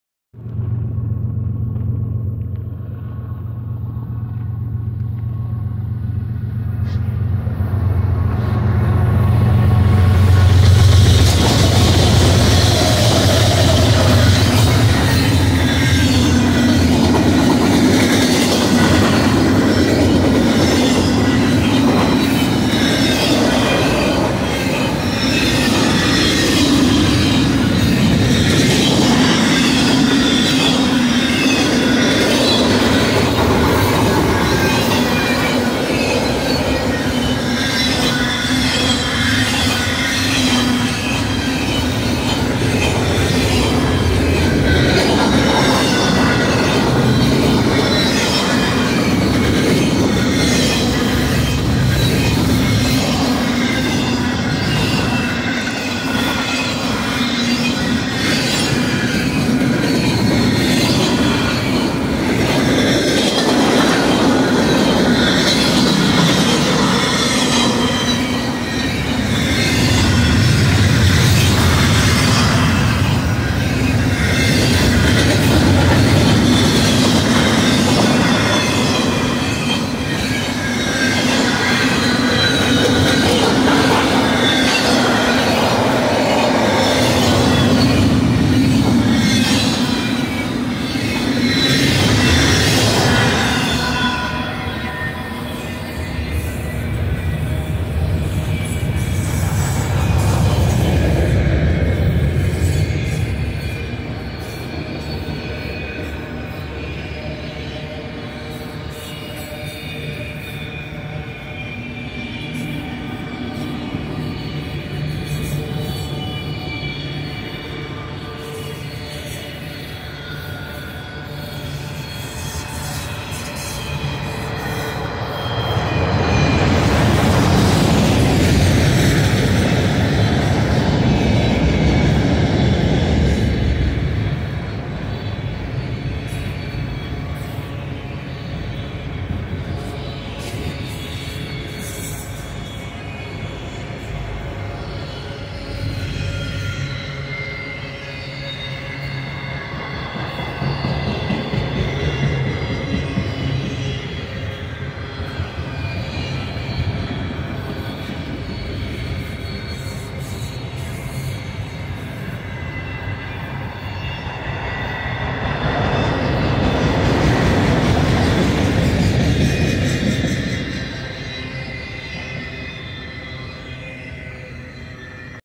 Duo BNSF Trains Meet At Daggett Ca!
The first one is a westbound BNSF autoracks train feat KCSM trail fail and the second one is a eastbound BNSF domestic intermodal/port stack train passing each other here at Daggett Ca!

Here you will see in this train video is the first BNSF autoracks train like autorack cars

Needles Sub Milepost 733.0 Mainline 1 First Lead Engines
7341-ES44DC (BNSF) (Leading Unit)
4718-ES44AC (GE) (KCSM) (Trailing)
7197-ES44C4 (BNSF) (Trailing)
1035-Dash-9C44-9W (BNSF) (Trailing)
Mainline 2 Second Lead Engines
7863-ES44DC (BNSF) (Leading Unit)
1096-Dash-9C44-9W (BNSF) (Trailing)
8359-ES44C4 (GE) (BNSF) (Trailing)